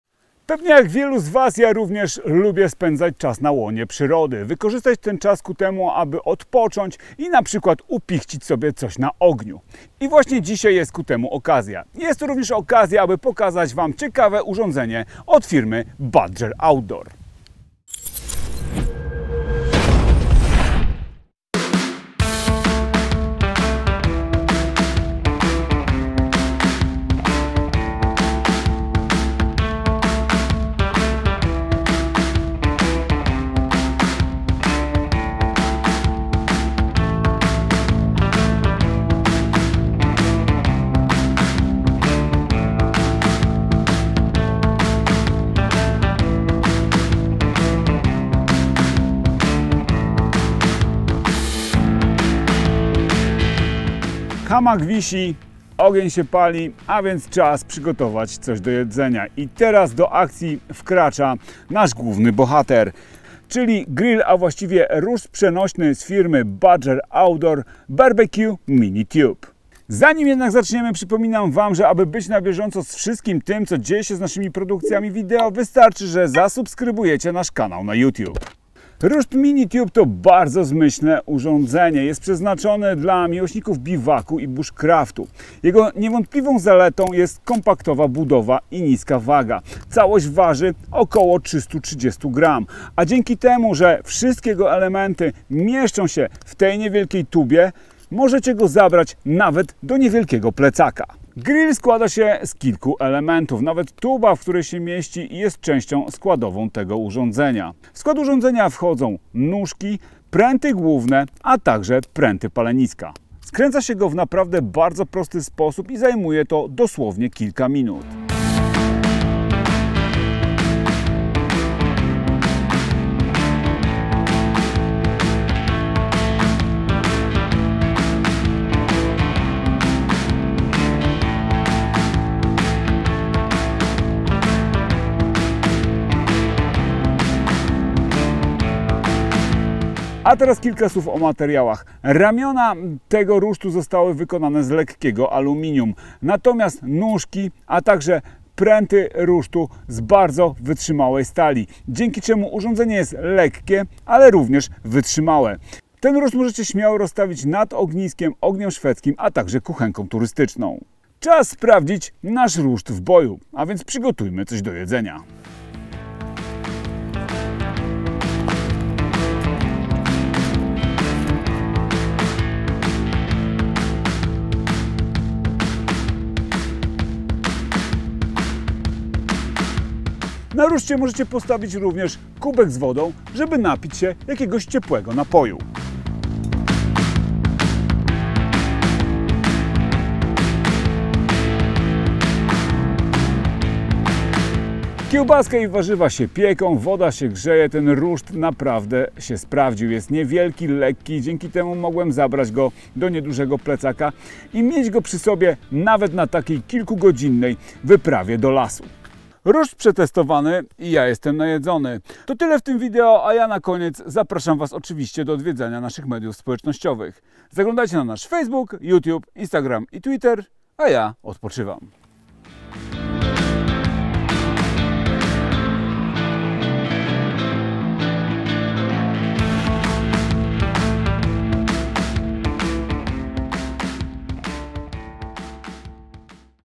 Grill turystyczny Badger Outdoor BBQ Mini Tube | Sklep Militaria.pl
Grill przenośny Badger Outdoor BBQ Mini Tube

Grill turystyczny, który może pełnić funkcję rusztu nad ognisko. Umożliwia wygodne grillowanie i gotowanie w terenie. Składana, kieszonkowa konstrukcja pozwala na spakowanie go nawet do jednodniowego plecaka. To sprawia, że grill jest dobrym wyborem dla miłośników bushcraftu, survivalu i outdooru.
Ramiona wykonane są z lekkiego aluminium, a nóżki i palenisko z mocnej stali. Dzięki temu grill jest jednocześnie lekki i solidnie wykonany.

Dane techniczne
Kolor: srebrny
Materiał: aluminium, stal
Wymiary
- złożony: 30,7 x 2,4 x 2,4 cm
- rozłożony: 30,7 x 15 x 27,5 cm
Wymiary rusztu: 27 x 26 cm
Waga: 330 g
Producent: Badger Outdoor